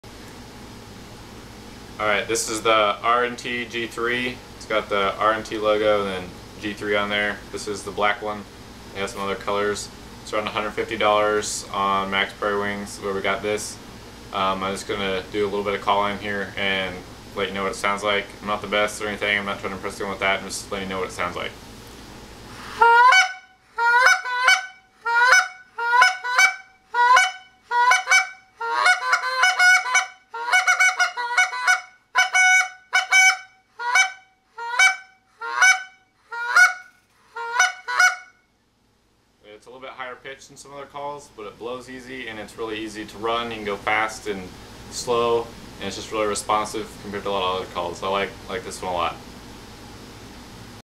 RNT G3 Goose Call
Quick overview and demo of the G3